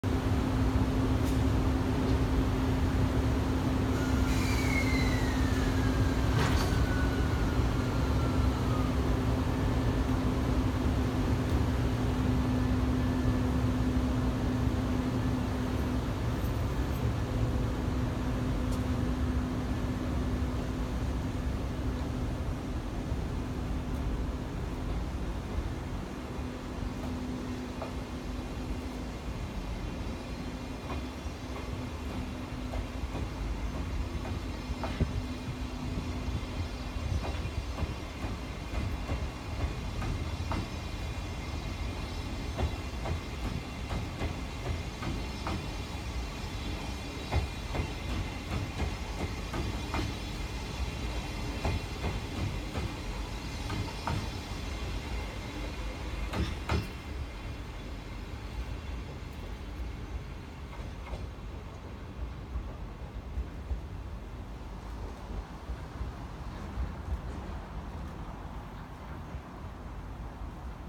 EF65 1135 サロンカーなにわ 全般検査明け 御着駅発車 2017/07/20
御着駅を発車する14系客車
全般検査明けのサロンカーなにわです